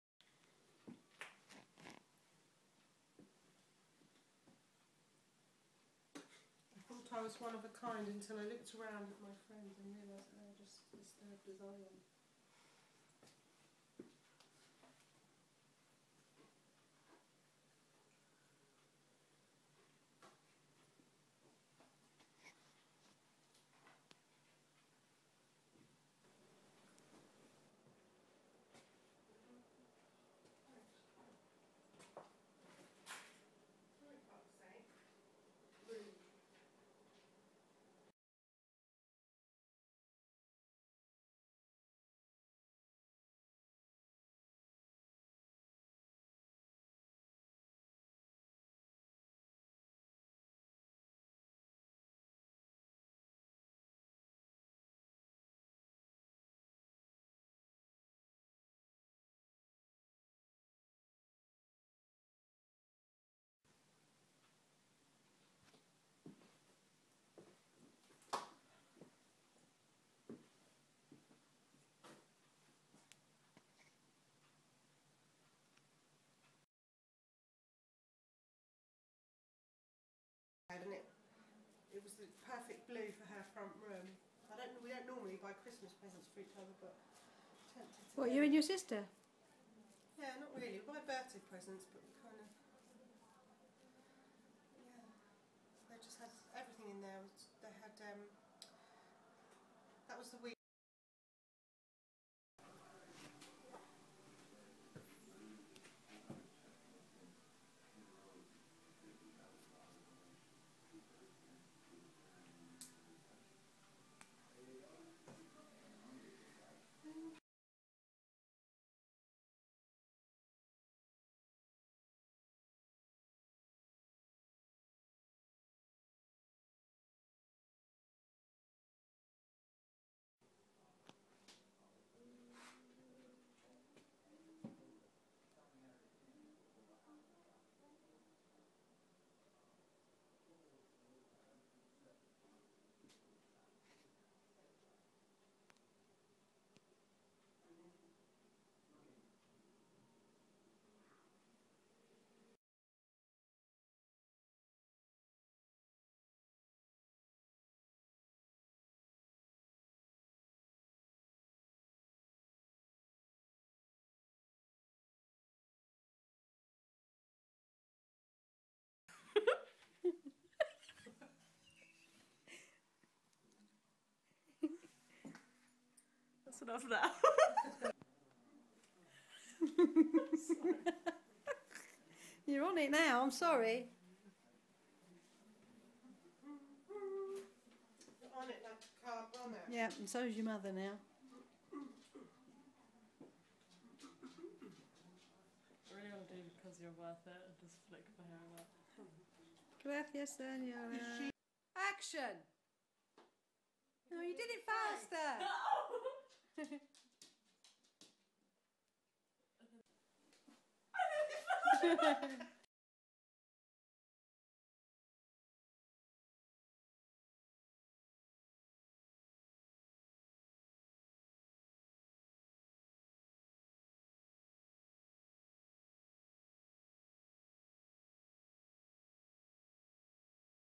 Two Tone Hair Transformation using Bleach and Permanent Medium Brown Tint.
Hello! This is a quick video showing a two tone colour change from blonde/orange to a golden blonde/medium brown.
After many colours including pink,Napalm Orange,black and blue...Alice has decided to go for a more natural pairing of colours.
All the products I used can be bought at Capital Hair and Beauty and Sally's Salon Supplies in the U.K.